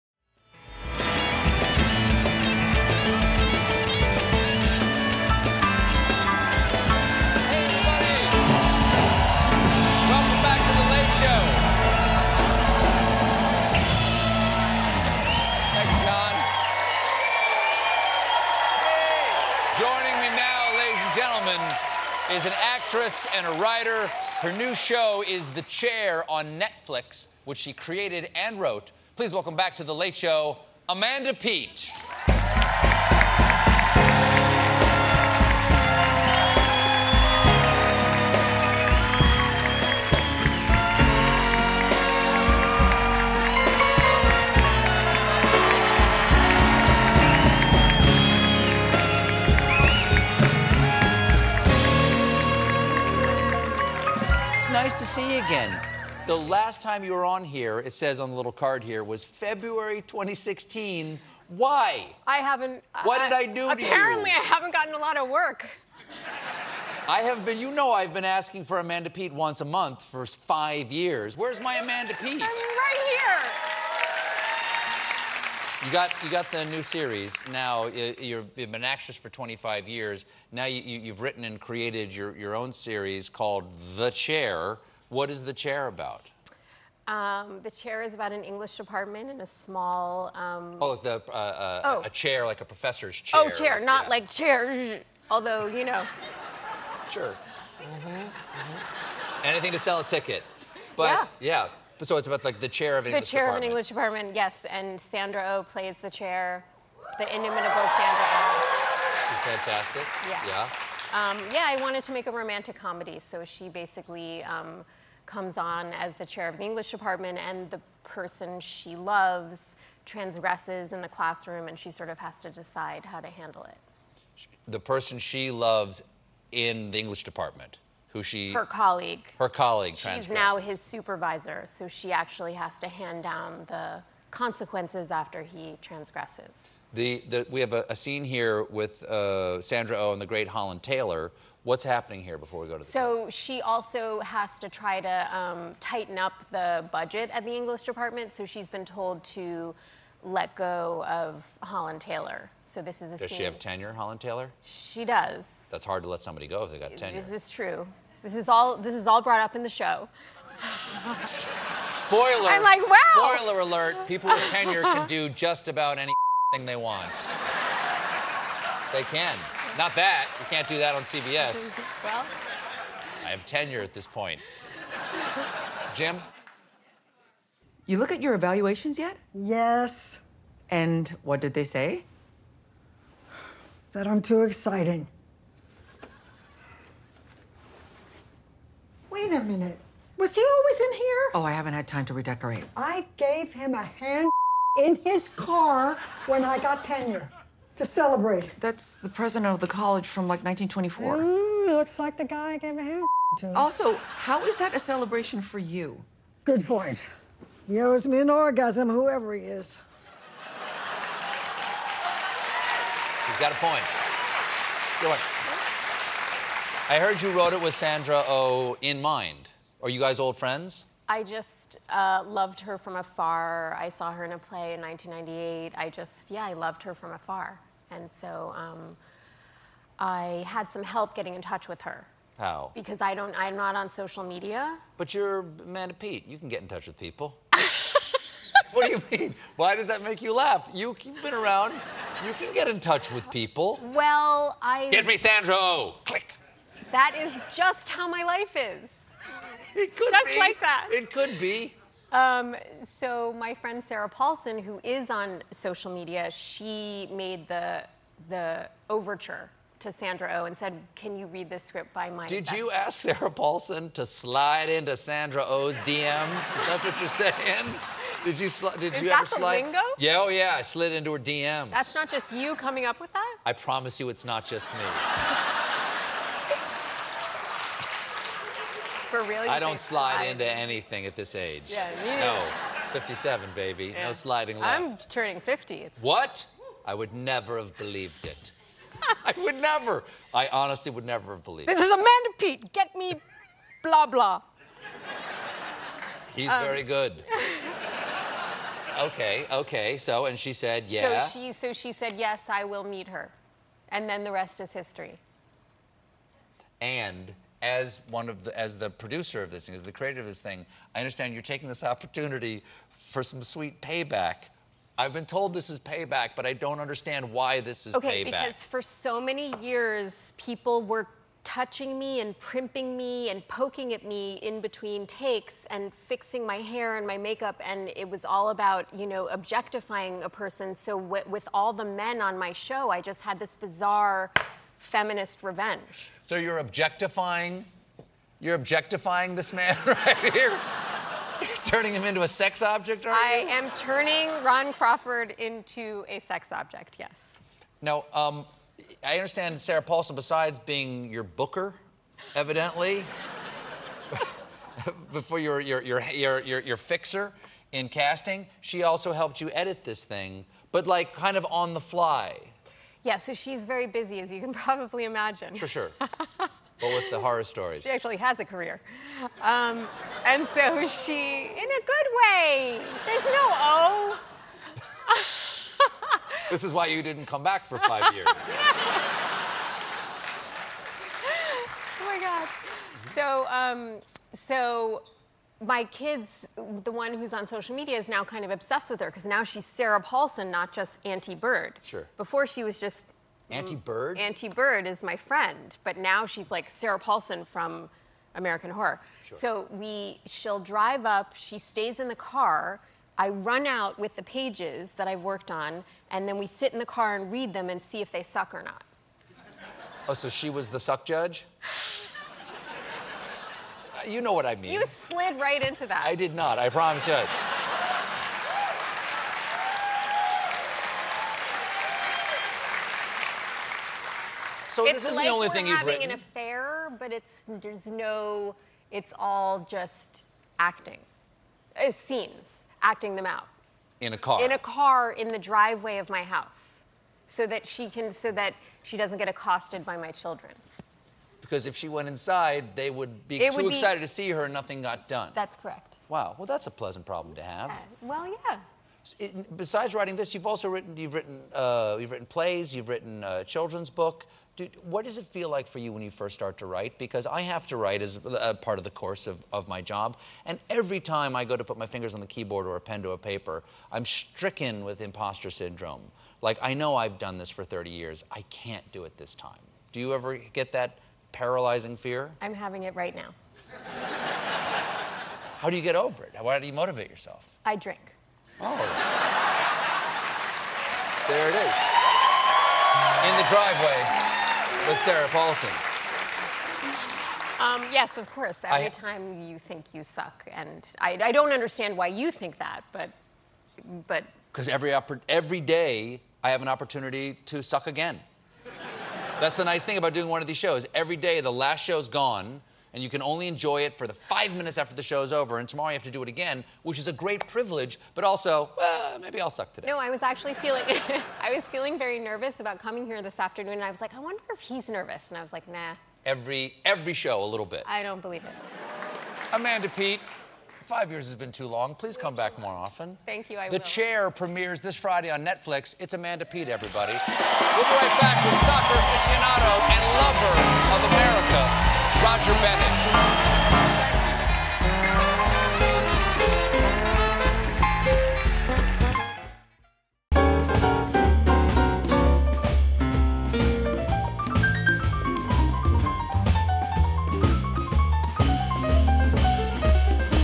Amanda Peet Got Sarah Paulson To Slide Into Sandra Oh's DM's While Casting For "The Chair"
Amanda Peet, who wrote and executive produced the new romantic comedy series "The Chair" on Netflix, called in help from her friend Sarah Paulson in order to convince the iconic Sandra Oh to play the lead.